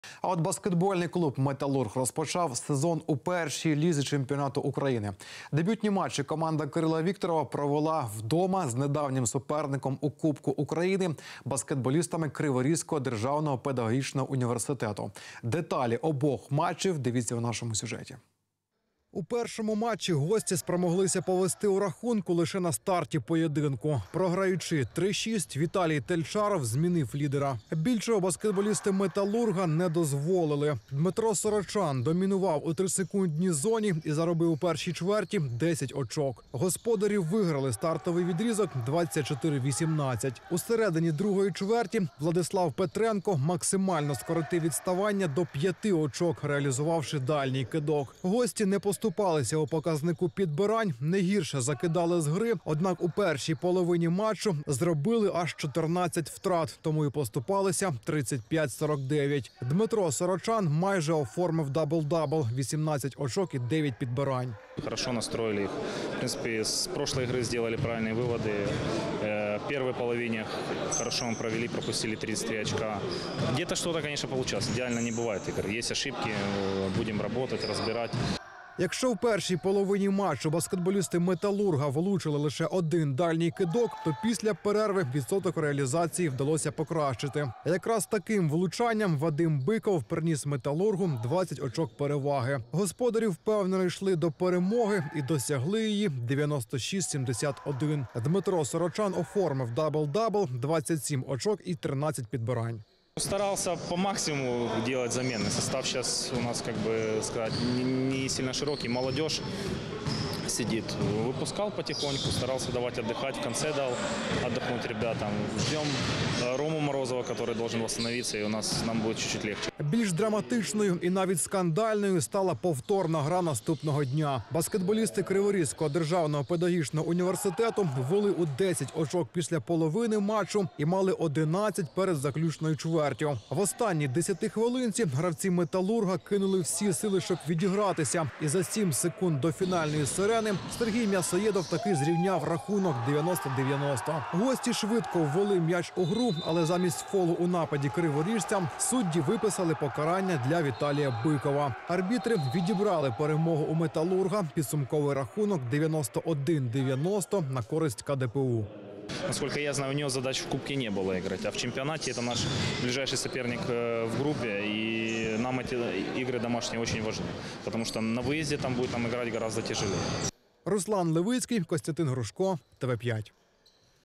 Сюжет TV5 про матчі БК "Металург" (Запоріжжя)
БК Металург (Запоріжжя) - БК КДПУ (Кривий Ріг) 90:91, 20171029
БК Металург (Запоріжжя) - БК КДПУ (Кривий Ріг) 96:71, 20171028